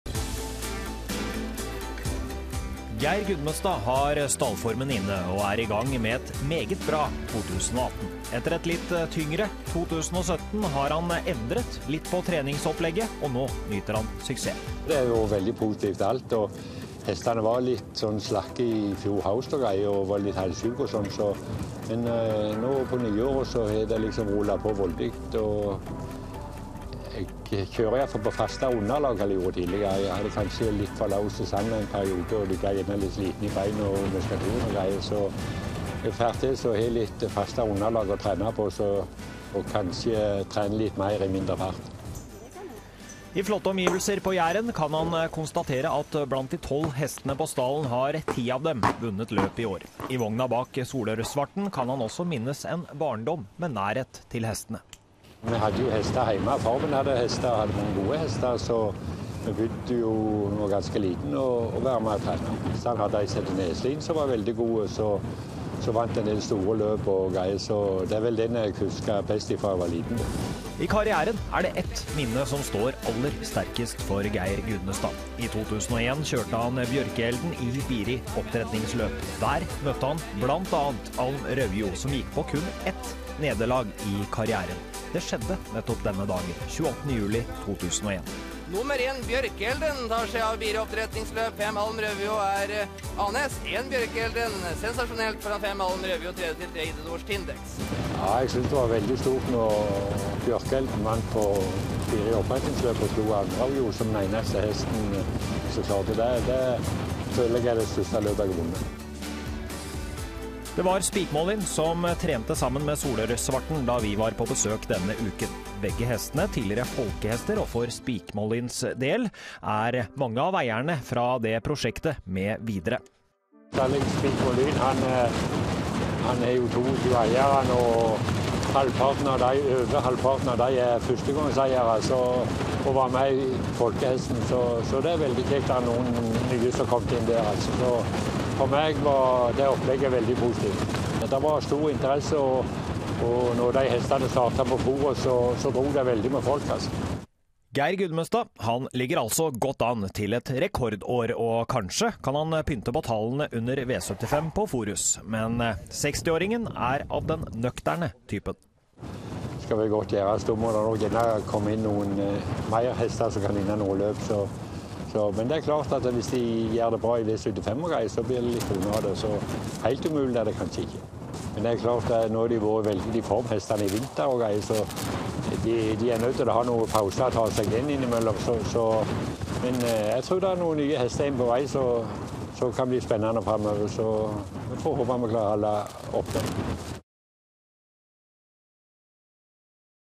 Geir Gudmestad med en kjempestart på 2018
Geir Gudmestad har gjort noen endringer i treningsopplegget og har startet 2018 på beste vis.